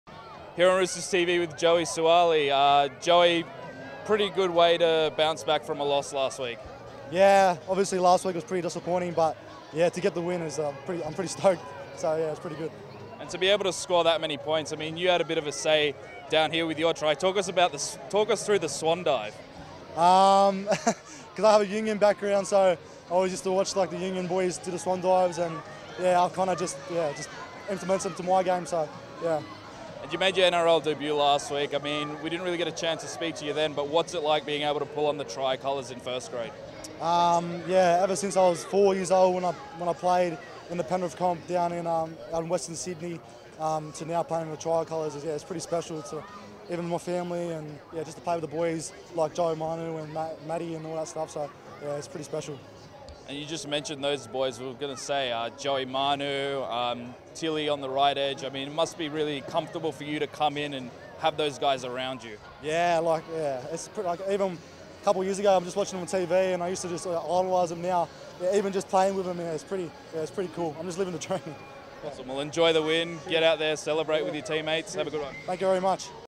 In the Sheds with Joseph Suaalii
RoostersTV spoke with Joseph Suaalii following a big performance against the Raiders on the Central Coast in Round 12